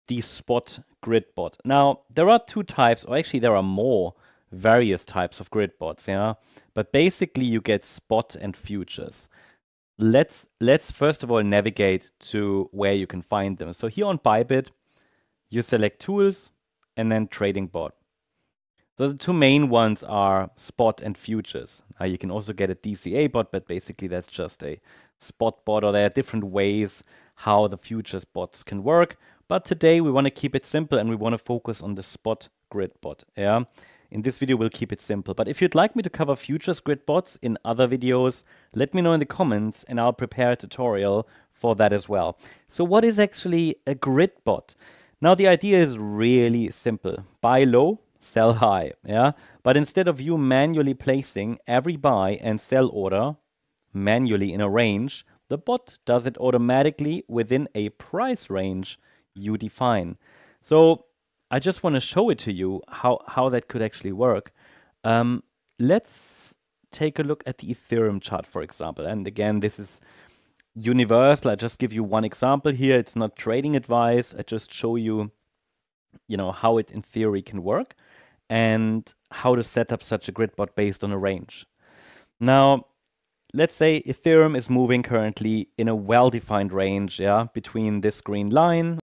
Spot Grid Bot Explained | Beginner’s Guide to Automated Crypto Trading (Bybit Tutorial)
In this video, I’ll show you how to set up and use a Spot Grid Bot in crypto trading, using Ethereum as an example.

Grid Bots are one of the simplest automated strategies: they buy low and sell high within a price range that you define. Instead of manually placing every order, the bot runs automatically 24/7, capturing profits from normal market fluctuations.

We’ll cover:

What a Spot Grid Bot is and how it works

Why it’s different from a Futures Grid Bot

Why Spot Grid Bots can be a useful risk management tool

How to choose a price range and why managing the bot is important

Step by step: how to set one up on Bybit

In this example, we’ll use Ethereum with a range from $4,060 to $5,000, based on current structure and Fibonacci support/resistance.

👉 Trade on Kucoin:
Receive a 20% fee discount:

👉 Trade with Trading212:
Receive shares worth up to $100: